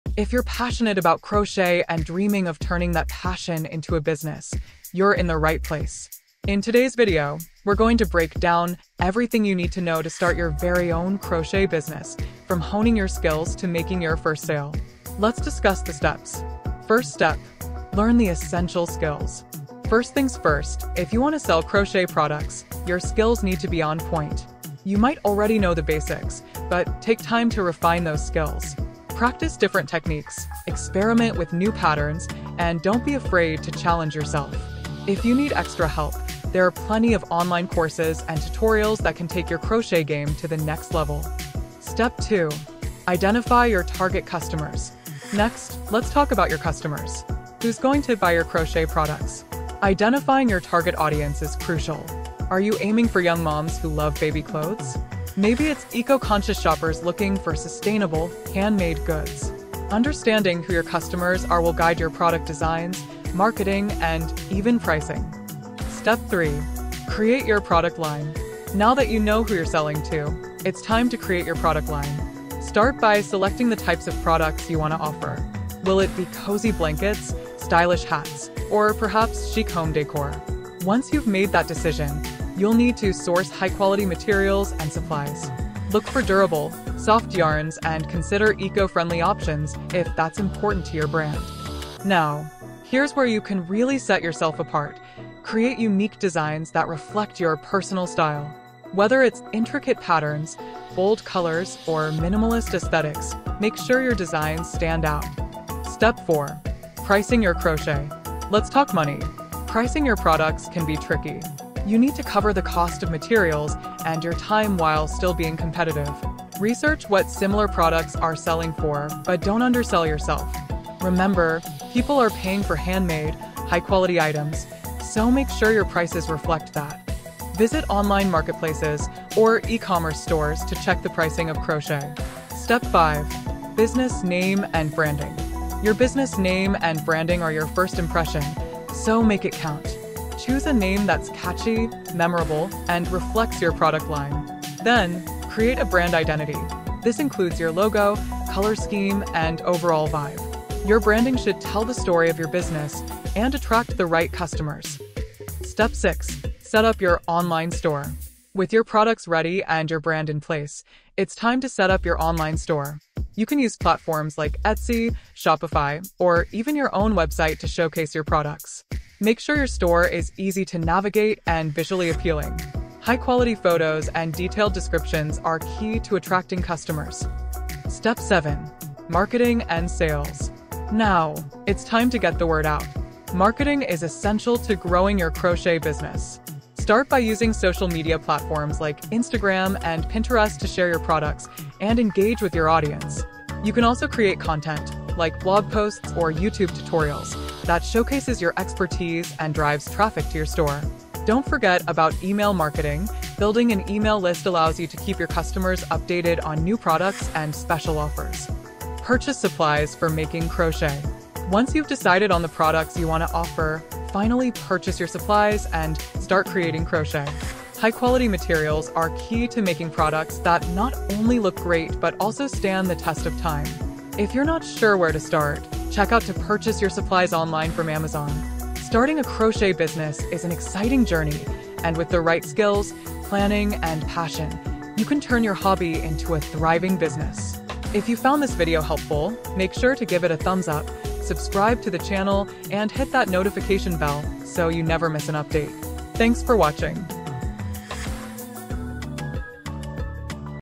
How to Start a CROCHET BUSINESS in 2024 - Step-by-Step Guide
Start a Crochet Business:

Ready to turn your crochet hobby into a thriving business? In this video, we'll walk you through every step of starting a successful crochet business, from honing your skills and identifying your target customers to setting up your online store and marketing your products. Whether you're just getting started or looking to take your craft to the next level, this guide has everything you need!

Steps to get started:

00:18 Step 1: Learn the essential skills
00:43 Step 2: Identify your target customers
01:07 Step 3: Create your product line
01:46 Step 4: Pricing your crochet
02:12 Step 5: Business name and branding
02:36 Step 6: Setup your online store
02:58 Step 7: Marketing and sales
03:30 Purchase supplies for making crochet
03:50  Final thought